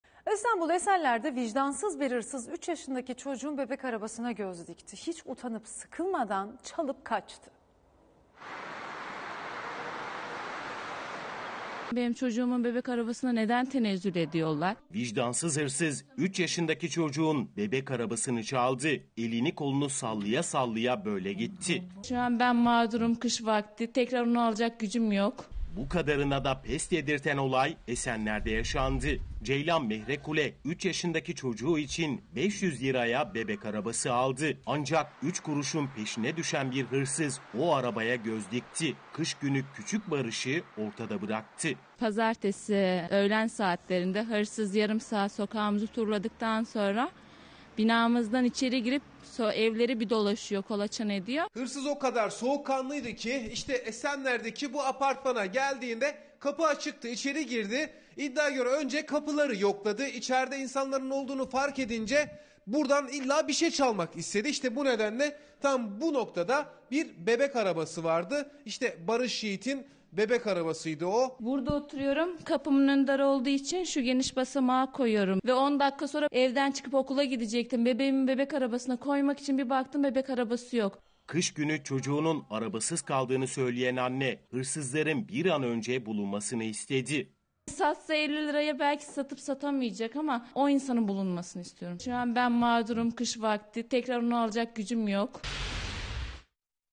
"Utanmazlığın bu kadarına pes" dedirtti!
İSTANBUL ESENLER'DE VİCDANSIZ BİR HIRSIZ 3 YAŞINDAKİ ÇOCUĞUN BEBEK ARABASINA GÖZ DİKTİ. HİÇ UTANIP SIKILMADAN ÇALIP KAÇTI.